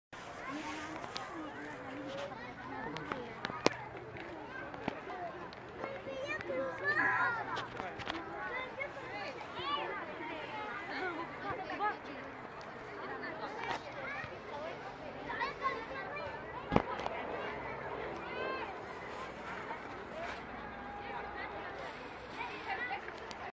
Mara Animals, Kuwait Zoo
(Record With Sony Ericsson Vivaz 720p ᴴᴰ)
I Thought it's Kind of Kangaroo But after I read the Information it Called Mara.